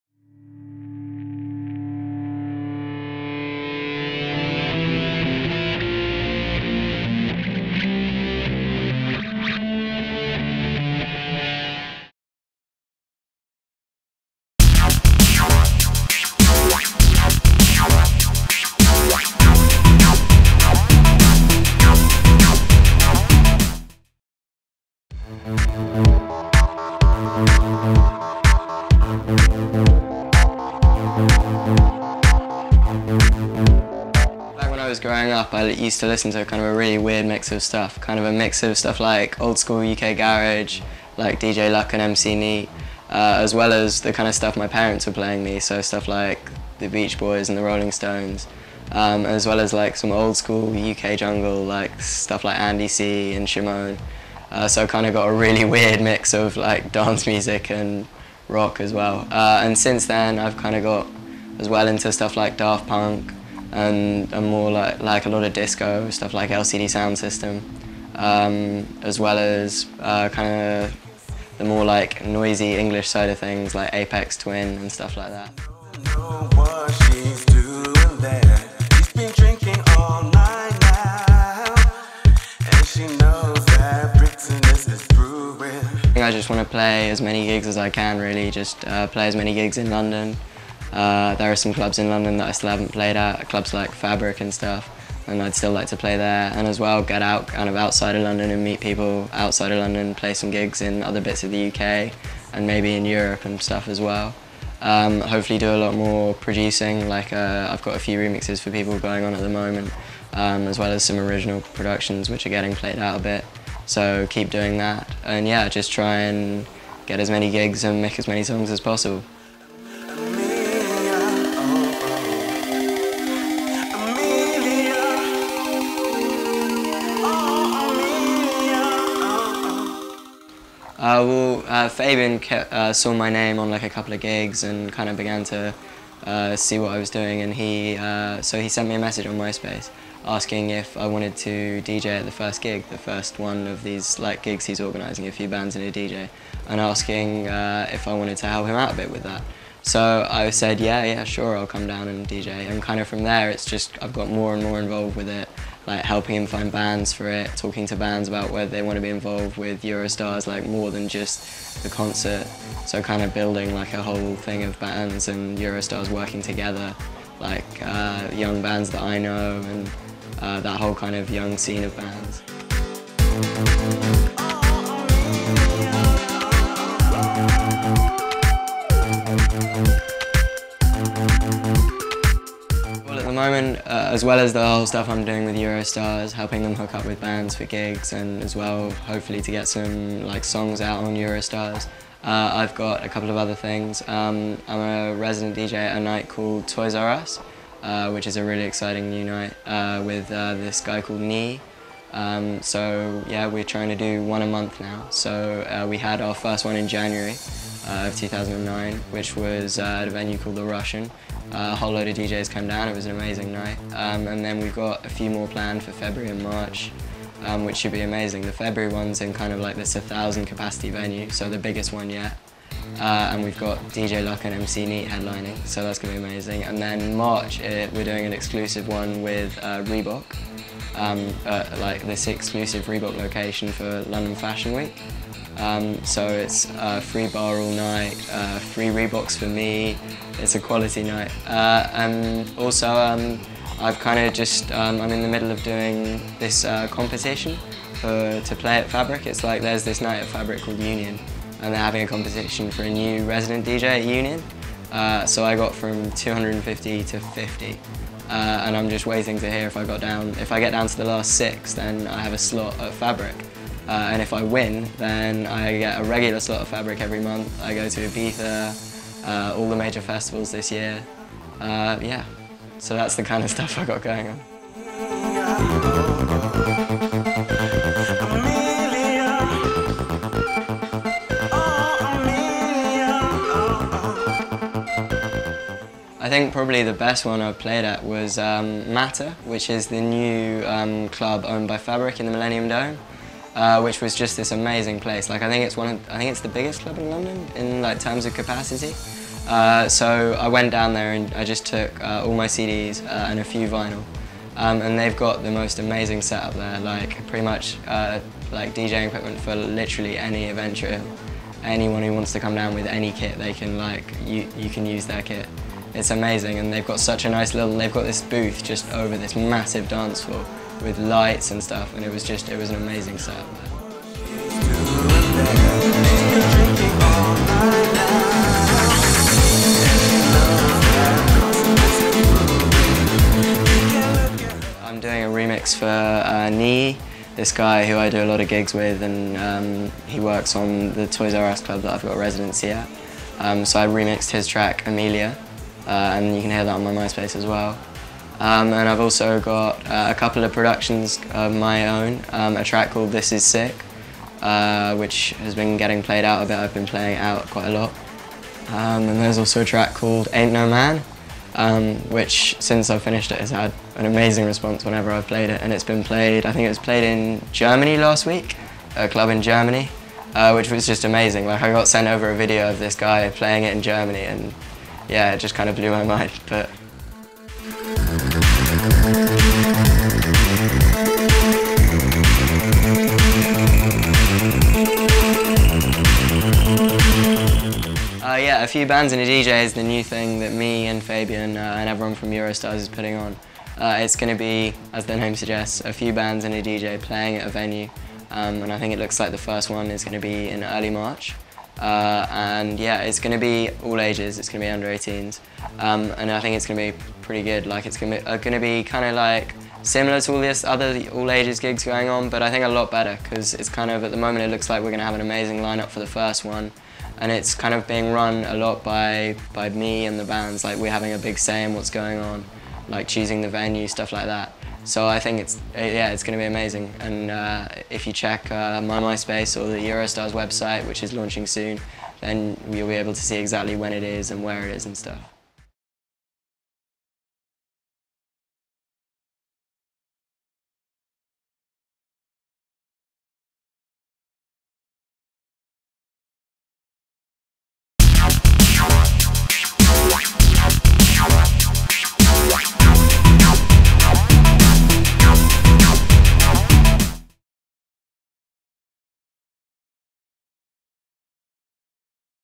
Ben White Interview HD
Catch an exclusive interview with DJ Ben White, a young, talented, emerging artist hitting the current club scenes showcasing his skills. He is currently working with Eurostars on some really great projects!